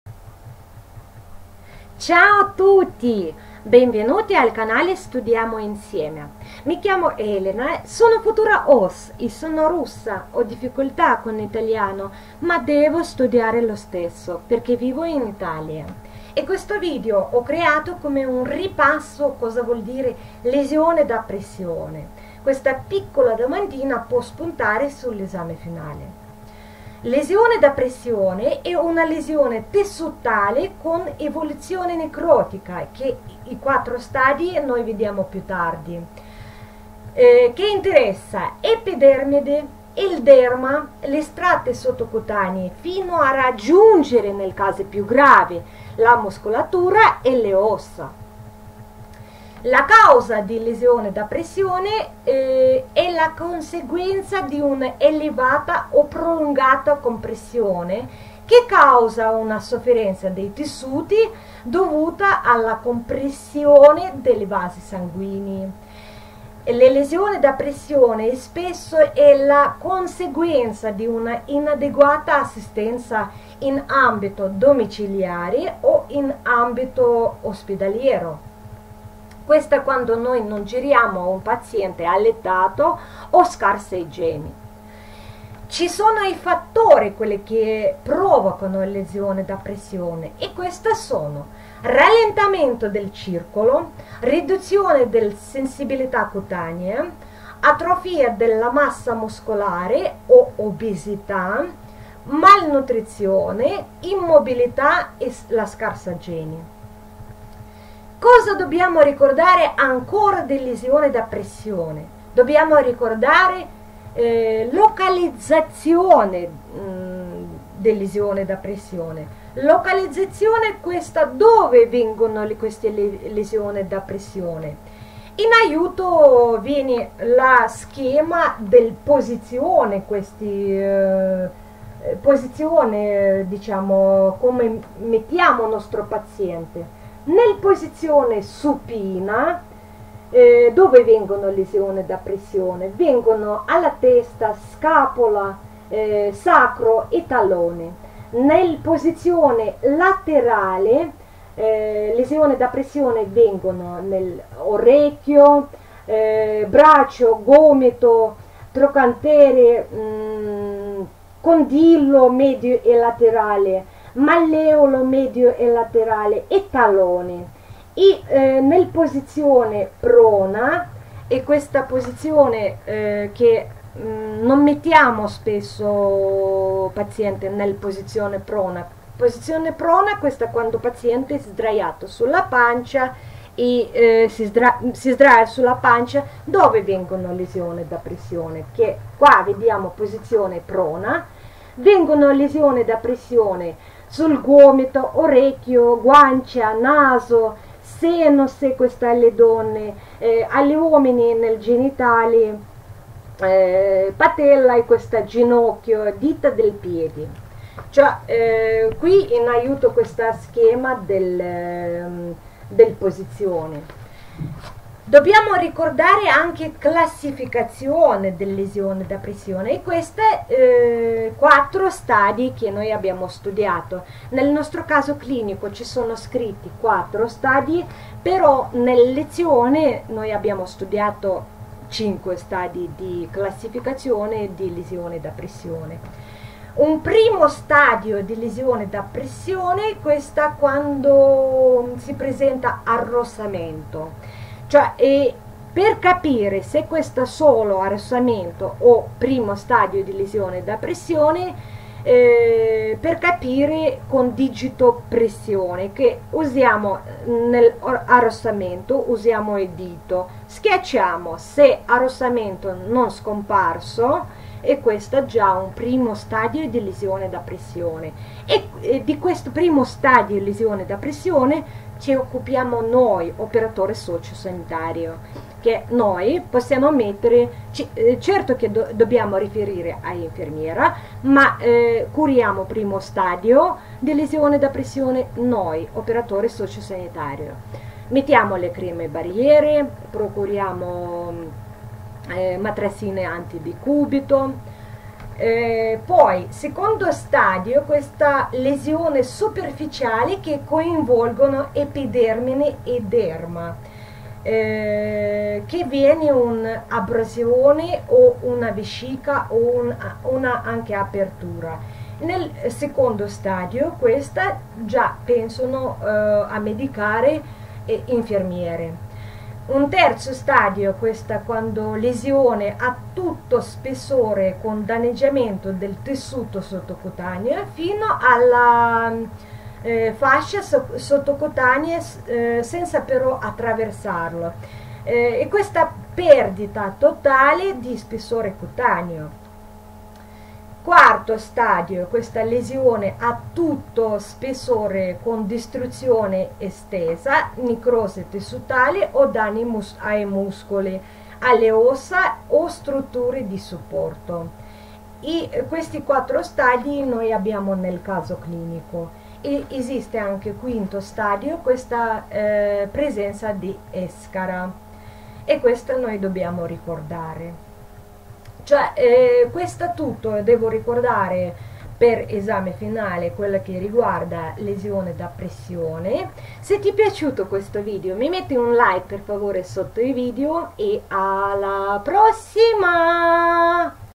LESIONE DA PRESSIONE (LDP). CLASSIFICAZIONE DELLE LDP e loro LOCALIZZAZIONE. CORSO OSS. Futuri OSS))
La lesione da pressione (comunemente detta piaga da decubito) è una lesione tissutale, con evoluzione necrotica, che interessa l'epidermide, il derma e gli strati sottocutanei, fino a raggiungere, nei casi più gravi, la muscolatura e le ossa. Piuttosto comunemente è detta anche "piaga".